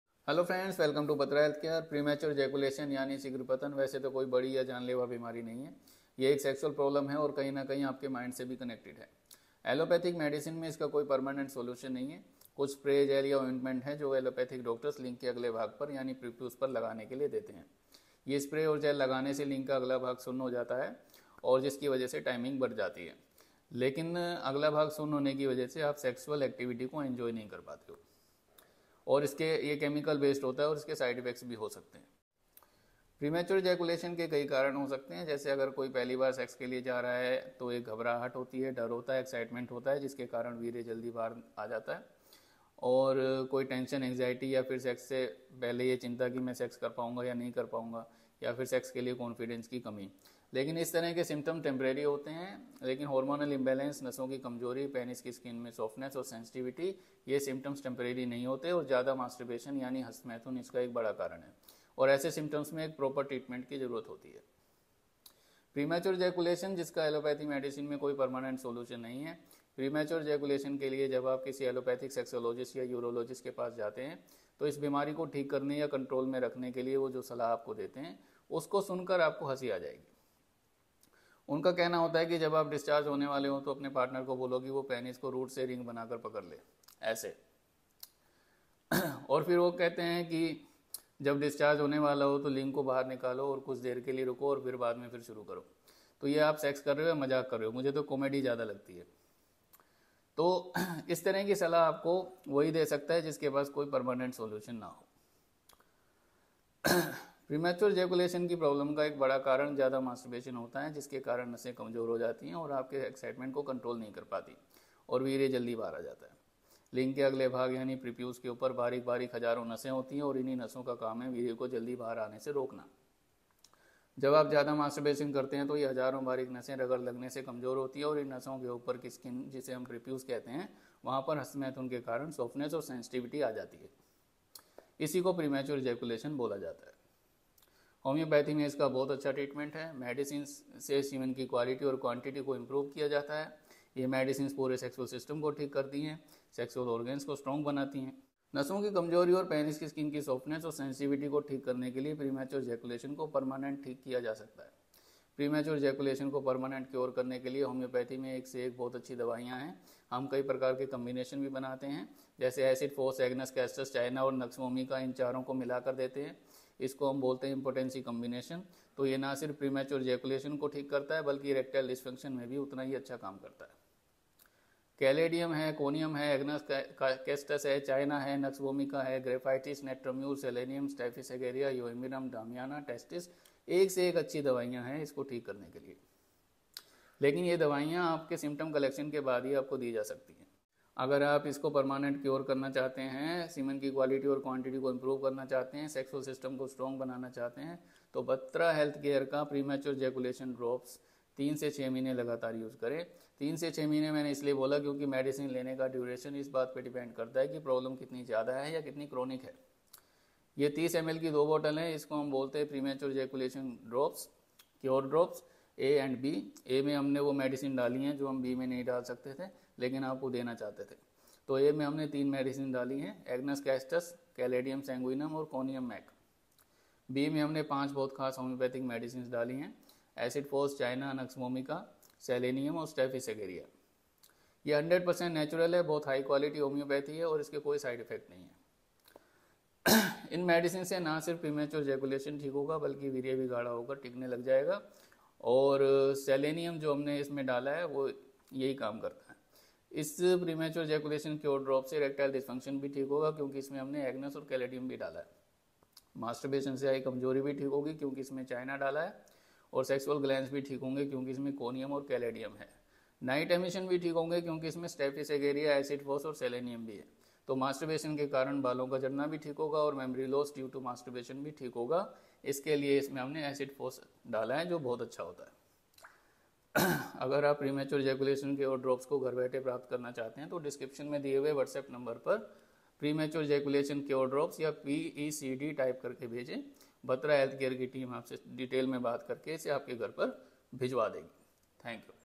premature ejeculation symptoms,causes and permanent treatment@BatraHealthCare
premature ejeculation causes
symptoms and permanent treatment
is video mai premature ejeculation ke karan , lakshan , aur ilaaj ke bare mein vistar se bataya gaya hai.

premature ejeculation cure drops ko ghar baithe prapt karne ke liye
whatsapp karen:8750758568

all over india mai cash on delivery par available hai